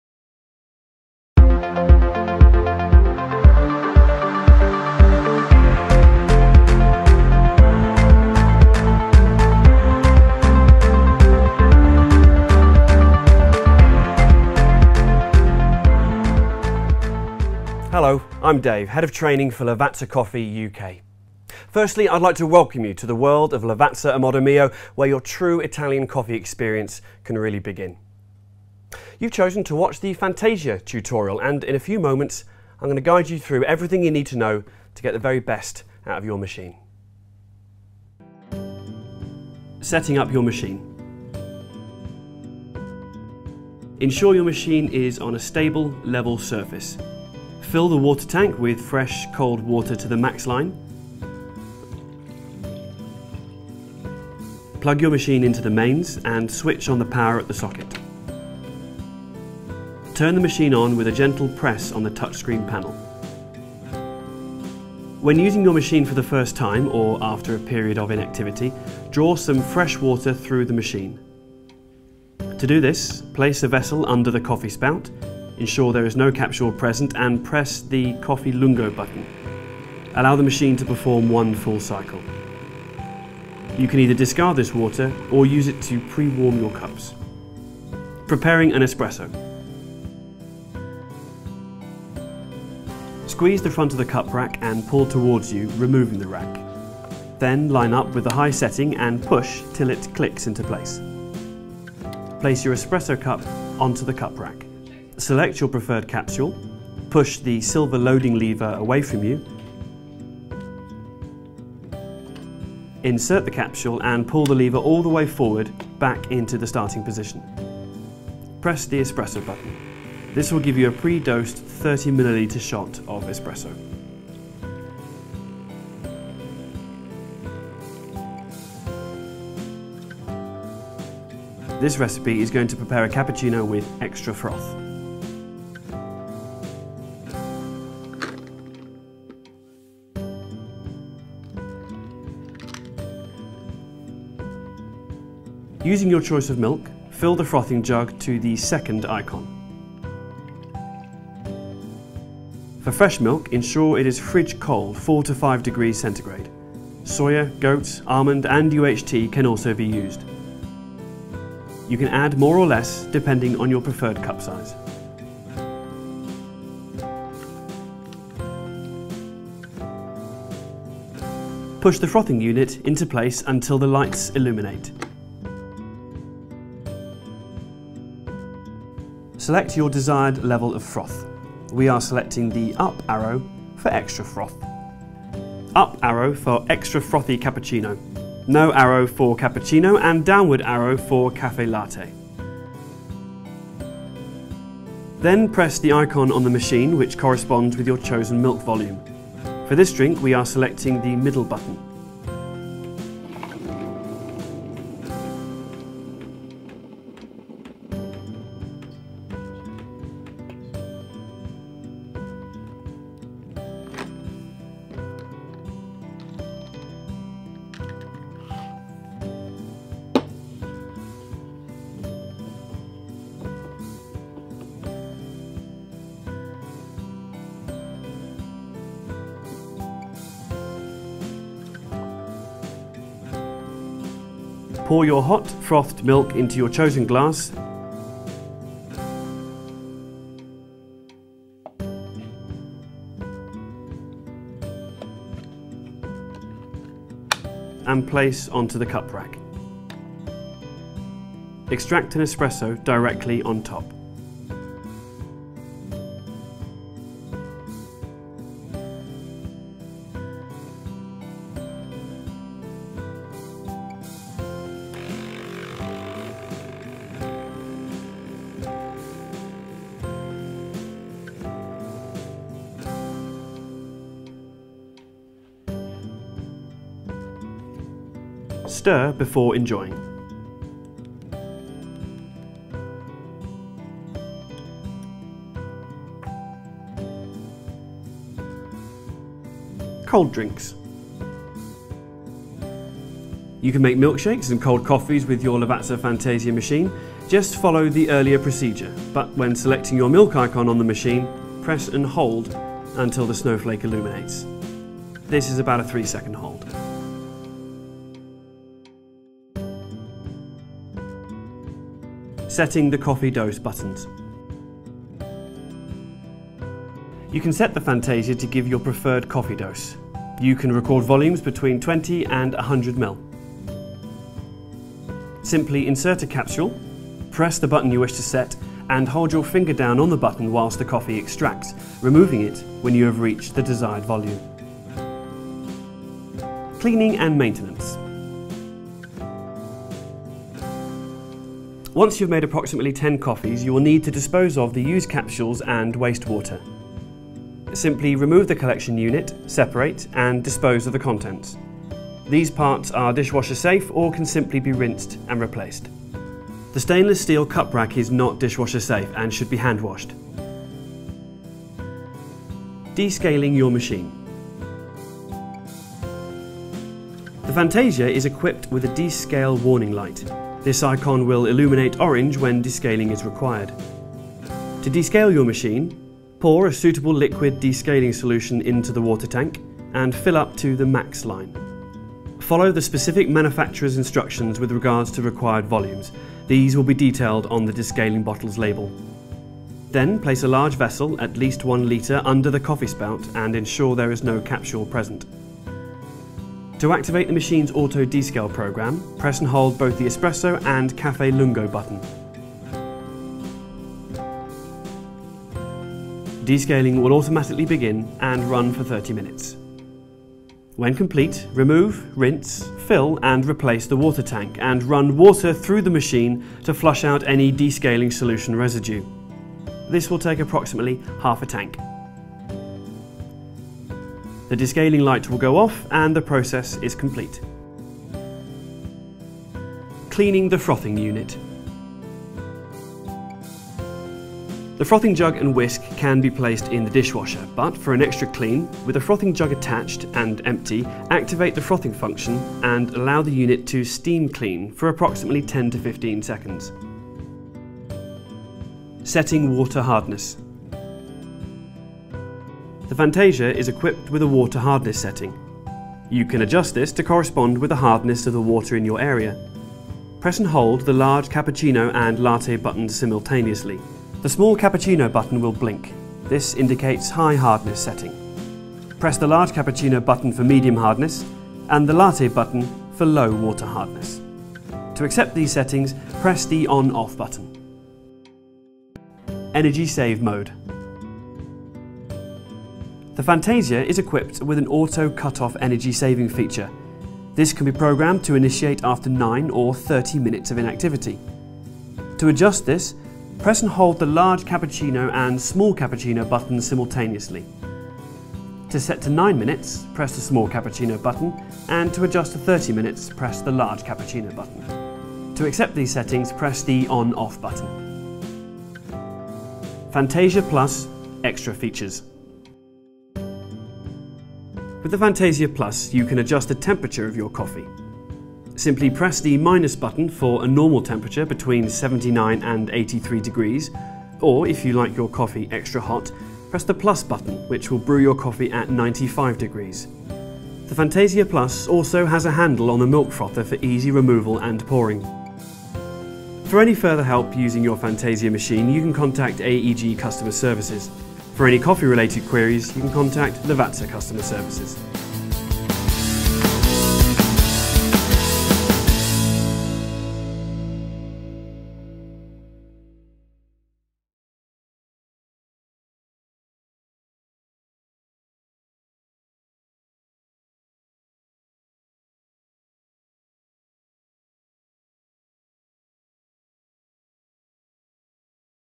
Lavazza Fantasia Coffee Machine
This video contains everything you need to know to get the very best out of your machine.

We all have many friends and family members who are fond coffee drinkers, which is why the Lavazza Fantasia Coffee Machine would make the perfect gift.

Ryman stocks a wide array of products to help you make delicious hot drinks! Click on the link below to view out Lavazza range.